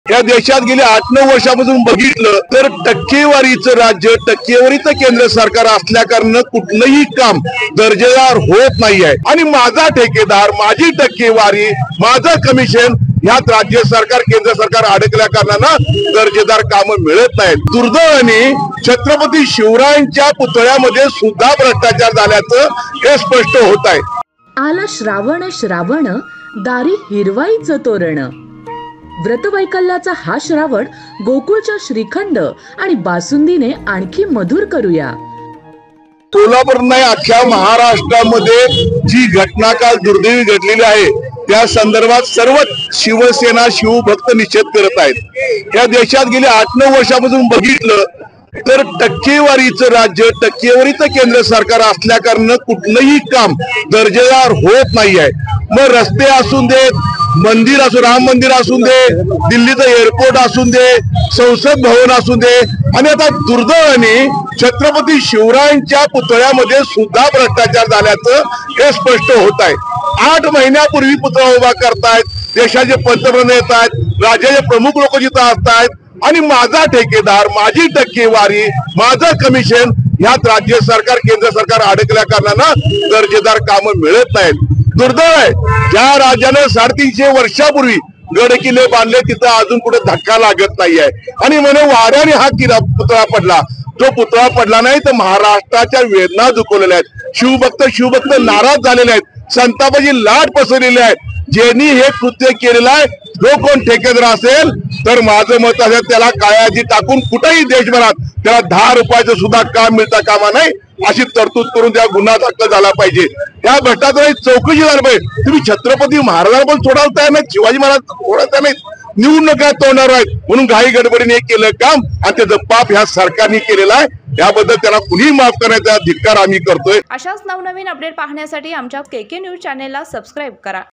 छत्रपती शिवरायांच्या पुतळ्याच्या घटनेबाबत शिवसेना ठाकरे पक्षाच्या वतीने कोल्हापुरात निषेध
Karbhar (कोल्हापुरी कारभार ) is a 24x7 news and entertainment
Marathi language.